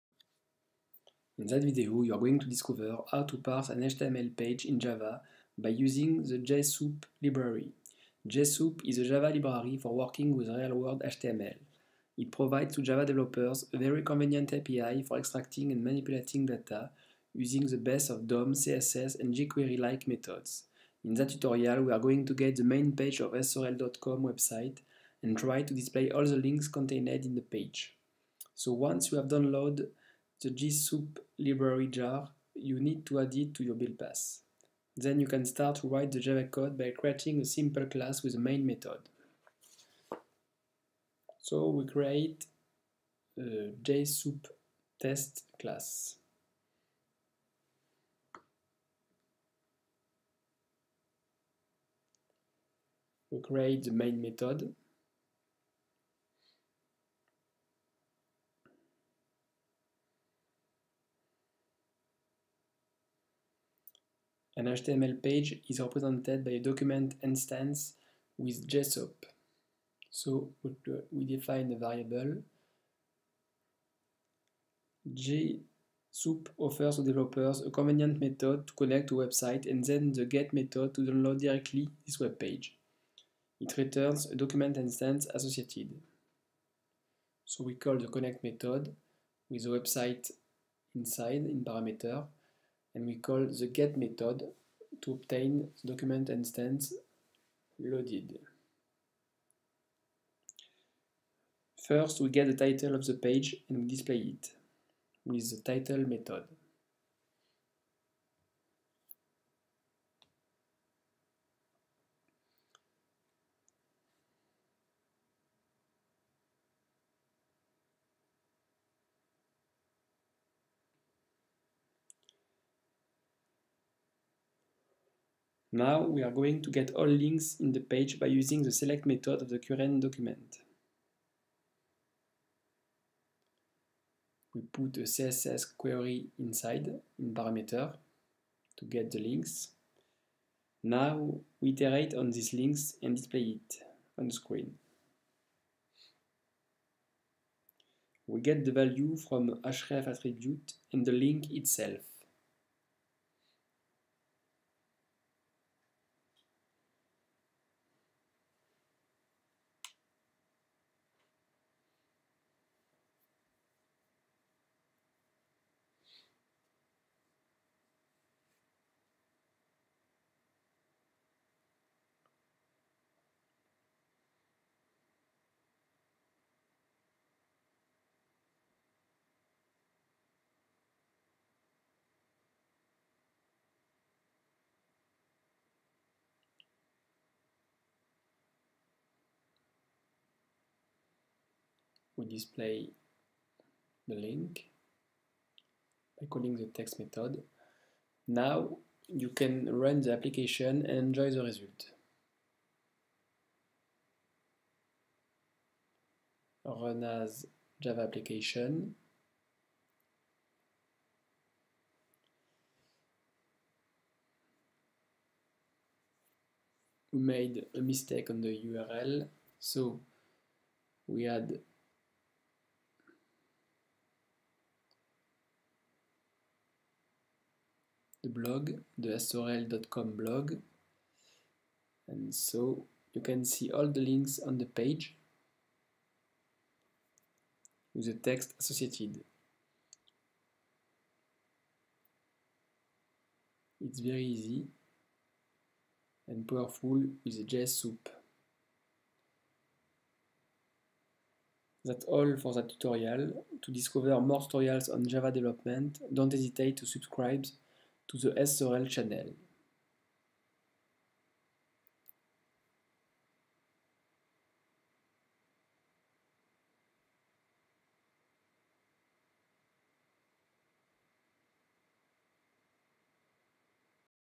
Parse an HTML page in Java with JSoup
Discover how to parse and HTML page in Java with JSoup Library.